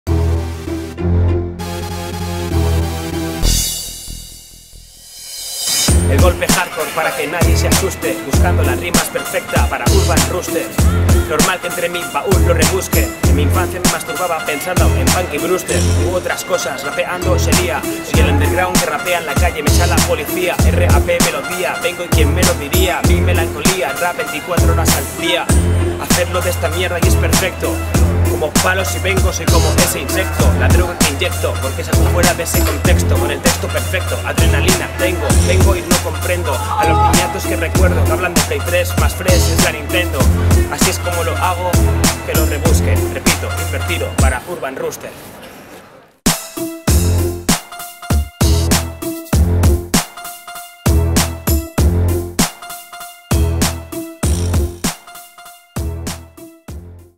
INVERT freestyle con The Urban Roosters #16
Os traemos al freestyler del momento. El hombre que ostenta el cinturón de campeón de la Red Bull Internacional. El hombre que no paró hasta conseguirlo. Endika Gutiérrez a.k.a INVERT nos trae esta improvisación desde Valencia, a las puertas del Natura Dub, donde la policía nos jodió la fiesta, pero no pudo callar el freestyle en la calle. Invert lo logró. Y tú, ¿tienes un sueño? Make It Real con Urbanroosters.com